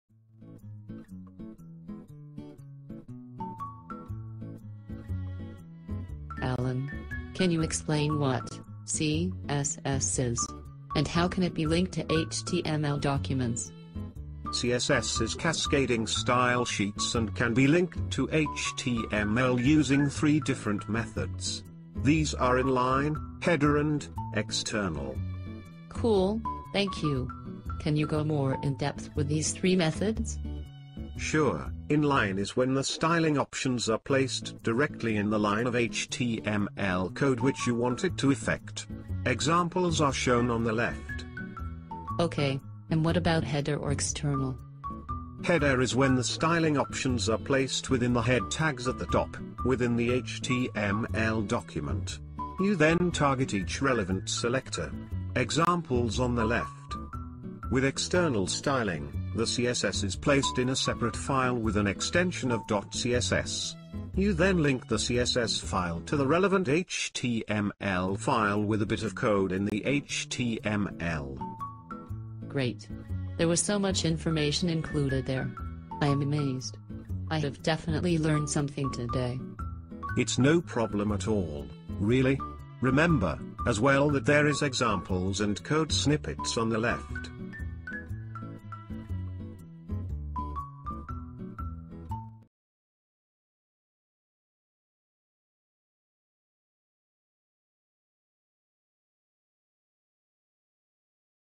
What is CSS and how can it be linked to an HTML document?
This short animation tells you what CSS is, the use for it and the three main methods you need to know in order to link it to an HTML document.

css,html,web,design,designing,internet,style